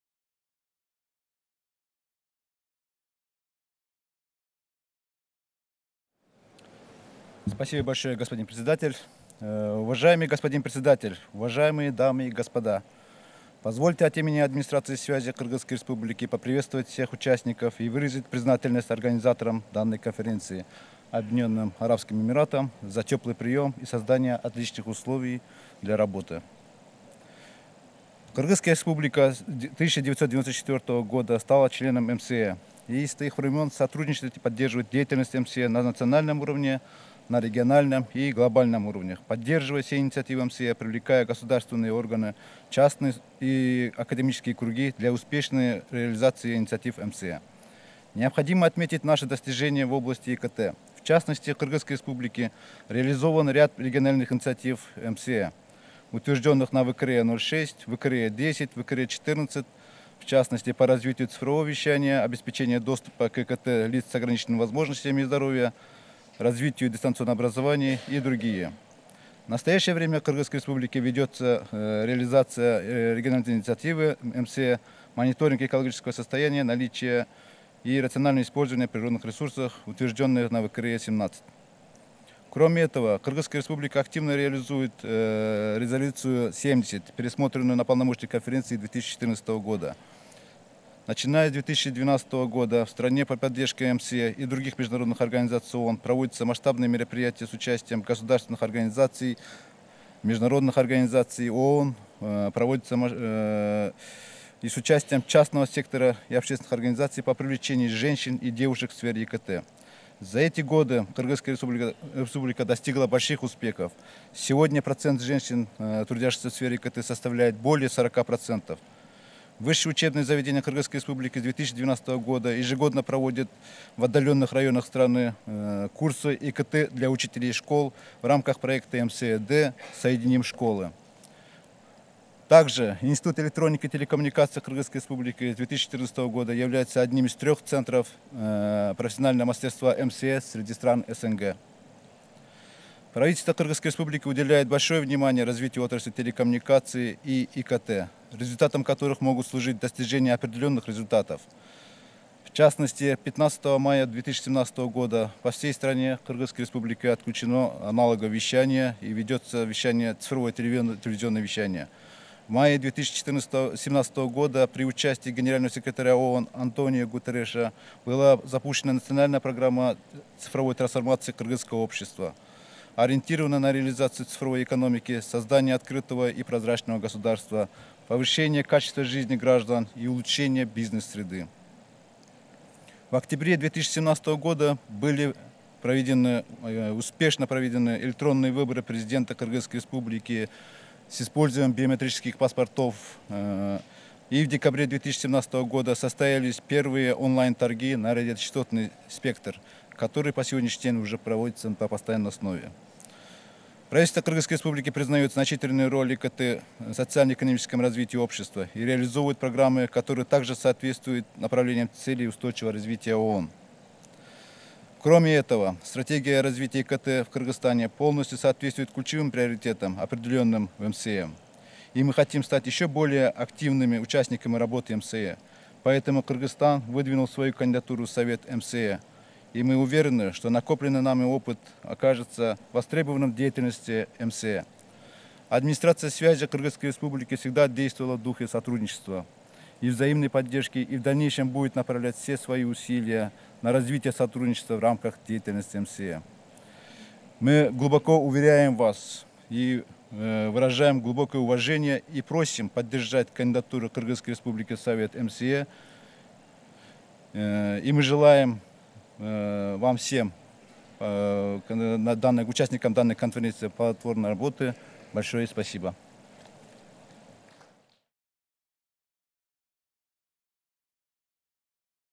PP-18: H.E. Mr Eshmambet Amatov, Vice Chairman, State Committee of Info Tech & Comms., Kyrgyzstan
Policy Statement PP-18 by H.E. Mr Eshmambet Amatov, Vice Chairman, State Committee of Information Technologies and Communications of the Kyrgyz Republic, Kyrgyzstan

Language: Russian